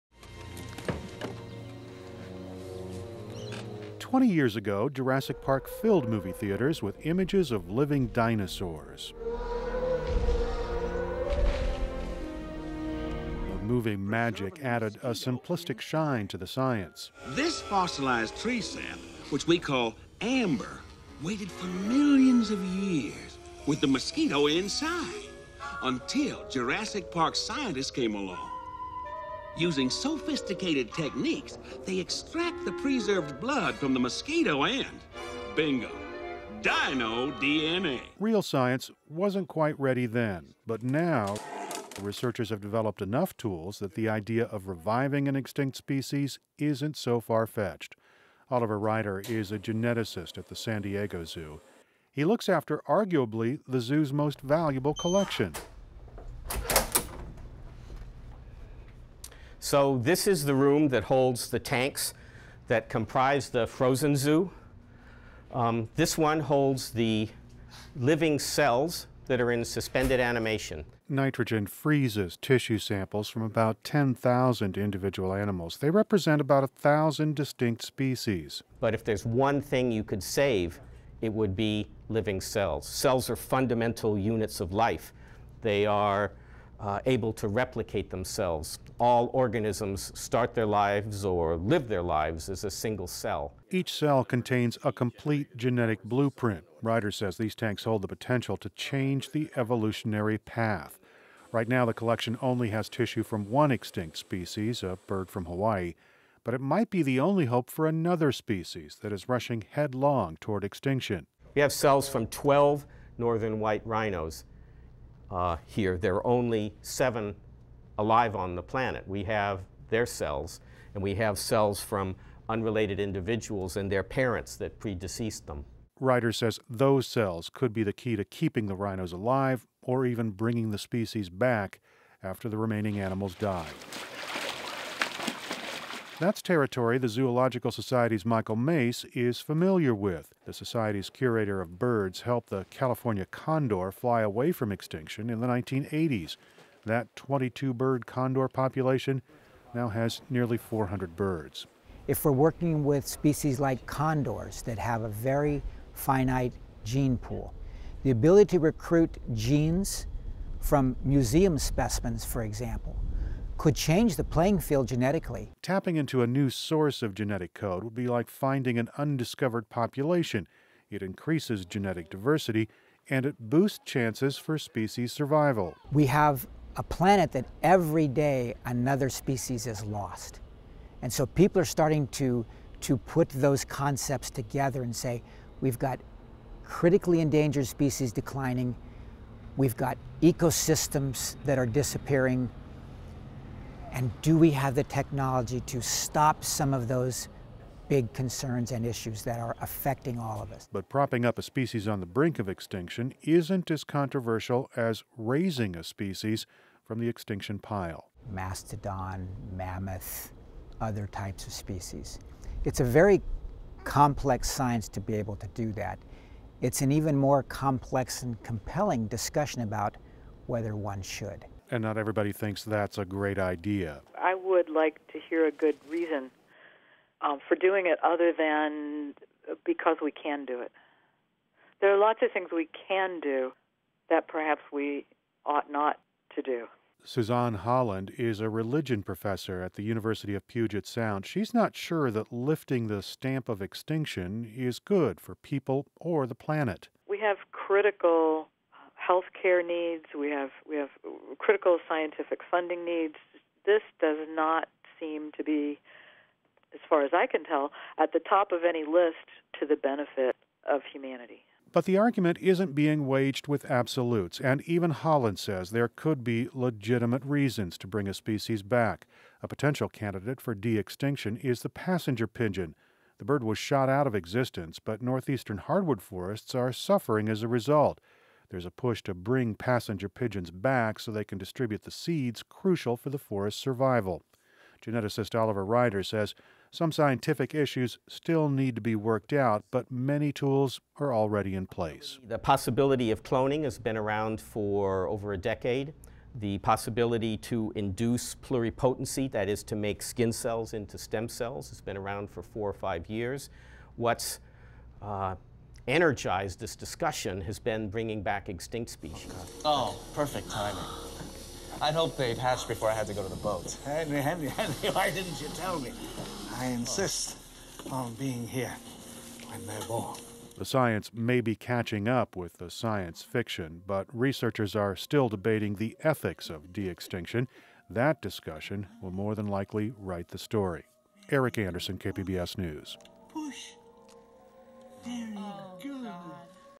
San Diego Researchers Discuss Bringing Back Extinct Animals
The fiction of Jurassic Park might be on the verge of reality. Scientists are talking about the possibility of bringing back extinct species.